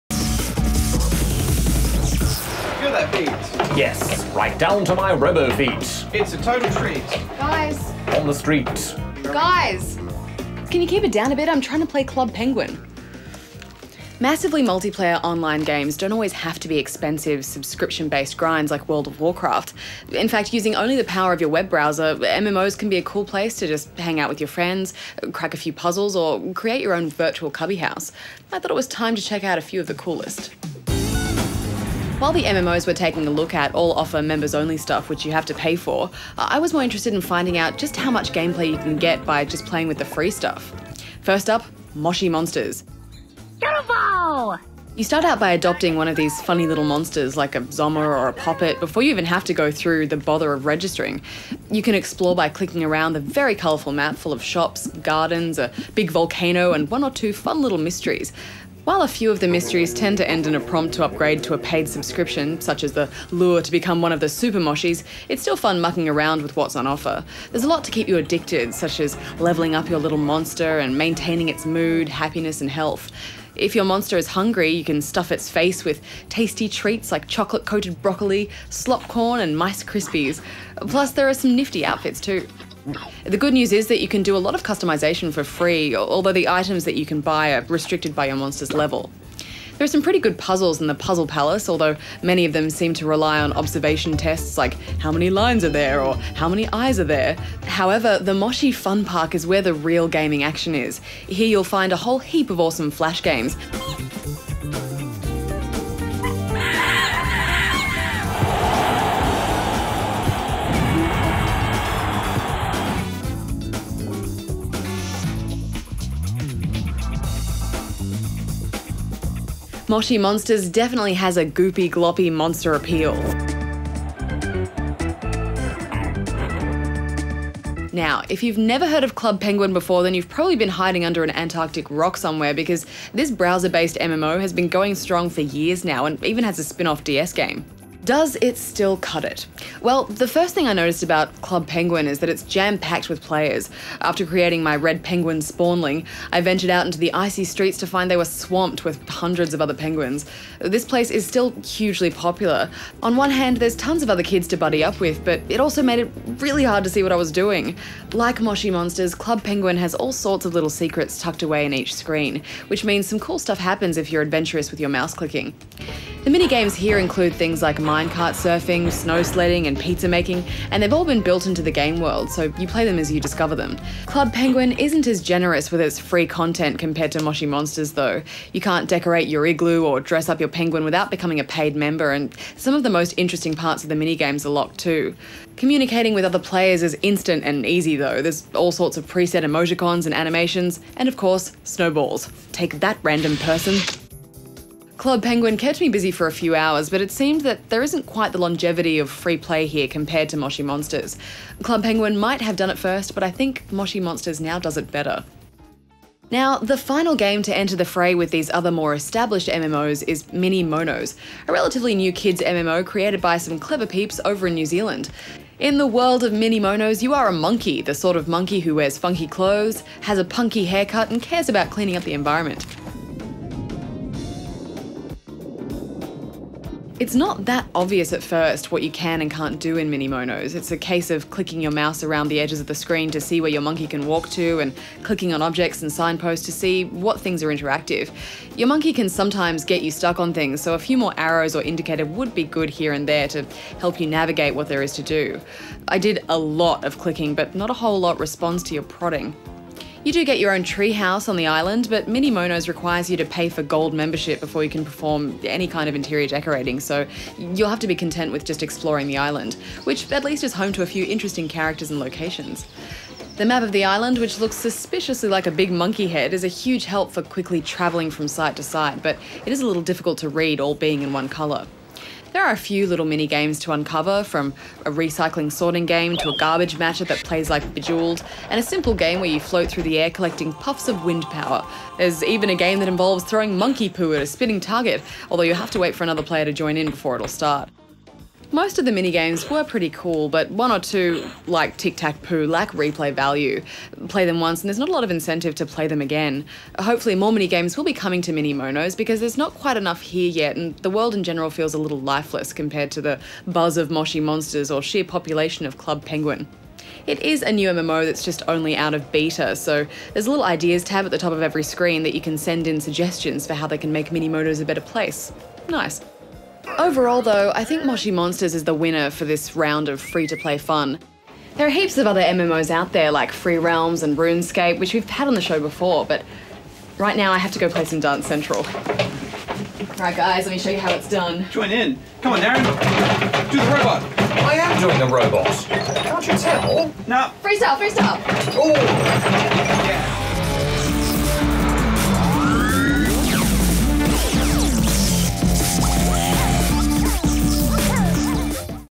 Moshi Monsters, Club Penguin, and MiniMonos | Game Review
Hex shows off some of the best free to play MMOs the internet has to offer.

--MORE SPAWN POINT--